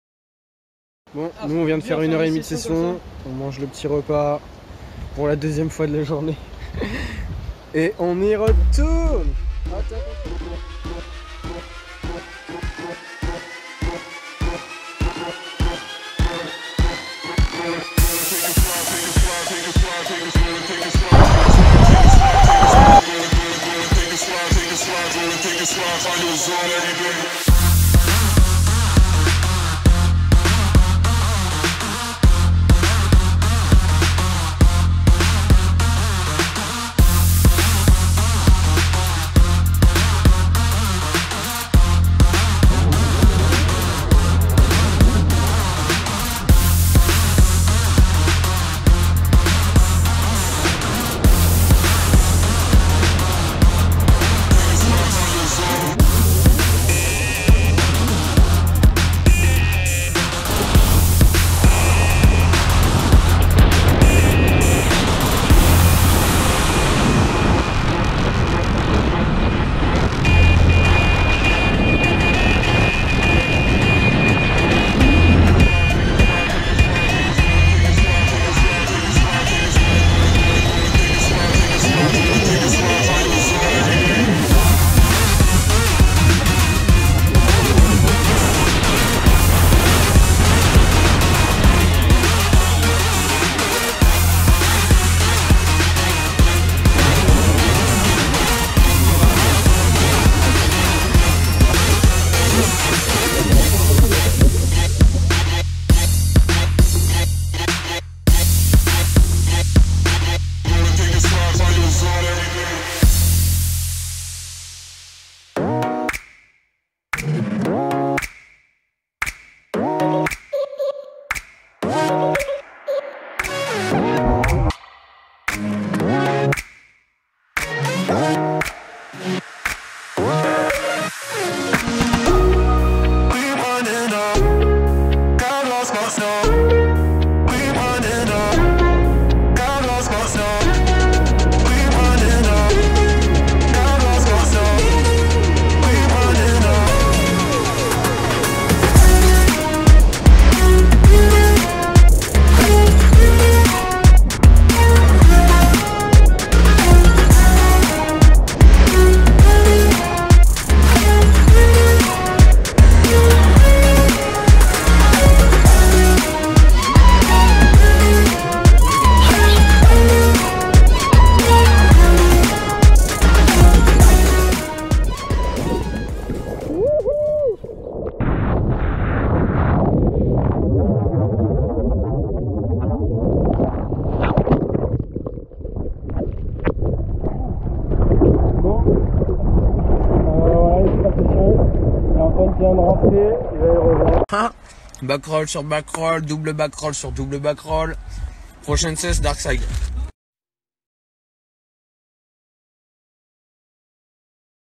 Crashs, Jumps, 39 km/h ! Summer Session Kitesurf - Pornichet/La Baule/Pont-mahé
📅 08/2020
📍 Loire-Atlantique
💨 +22 knots
Shot by gopro 8
Kite : 10m²

2 jours de navigation sur les spots de Loire-Atlantique : Pornichet et Pont-Mahé.
Test des données GPS de la gopro 8.